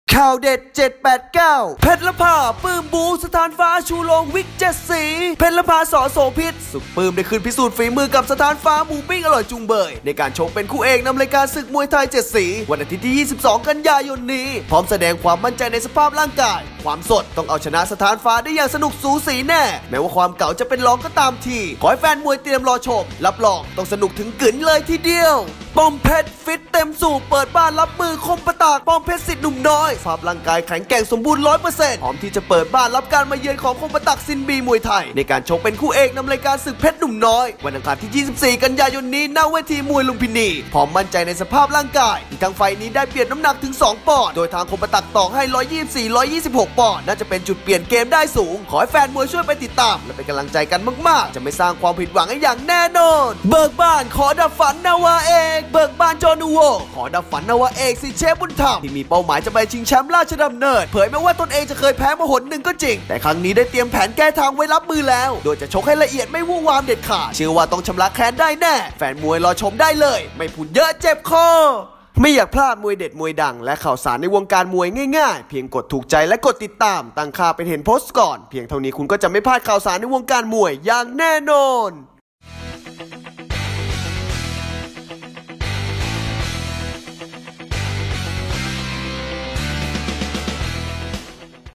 เพชรรภา ปลื้มบู๊ สะท้านฟ้า ชูโรงวิก7สี | มวยเด็ด789
เพชรรภา ปลื้มบู๊ สะท้านฟ้า ชูโรงวิก7สี

ข่าวเด็ดข่าวดัง ที่ใช้แต่หูฟังไม่ต้องใช้ตาอ่าน
ประจำวันที่ 14-15 กันยายน 2562 อัพเดทข่าวสารวงการมวยทุกซอกทุกมุมแบบกระชับฉับไว ฟังง่าย ไม่ต้องเสียเวลาอ่าน โดยทีมงาน